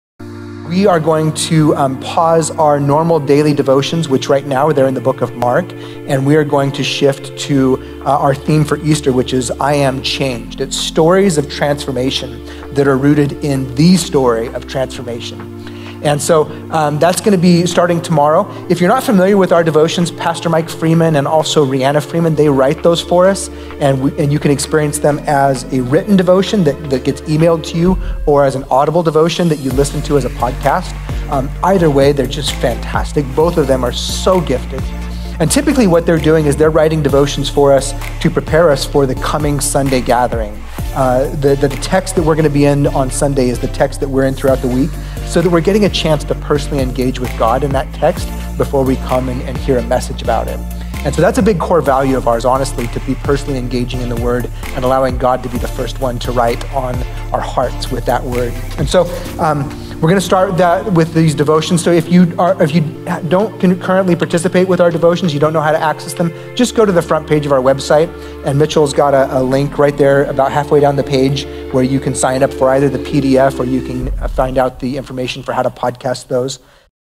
Easter Devotions
As we get ready for Palm Sunday and Easter Sunday, we invite you to engage in our devotions specially written around the theme: I. Am. Changed.

Sign up to get the devotions sent straight to your inbox at VineyardBoise.org/Devotions or listen to the devotion podcast on Spotify, Apple & Google Podcasts!